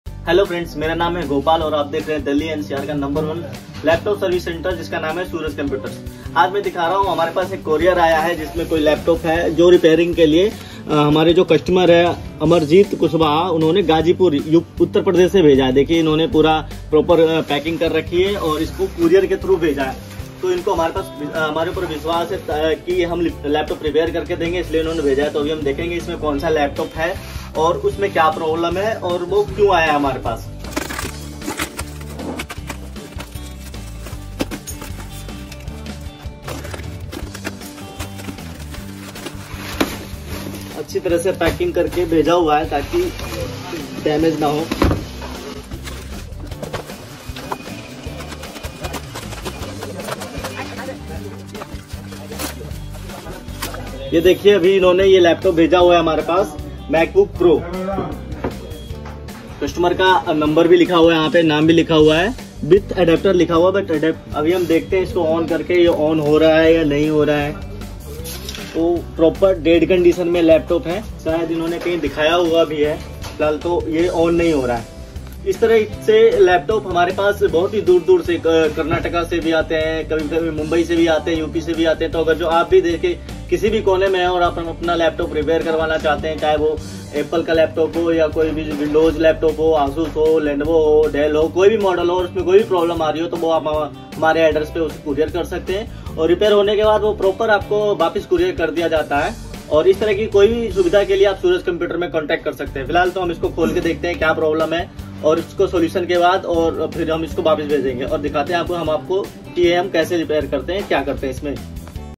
MacBook Laptop Sent for Repair by Courier: What to Expect! 🚚💻
Curious about the repair process when sending your MacBook for service by courier? Join us as we delve into the journey of a customer who sent their MacBook for repair. From packaging tips to tracking the repair progress, we cover it all. Gain insights into the steps involved and what you can anticipate when entrusting your MacBook to a repair service through courier. Watch now for a stress-free repair experience!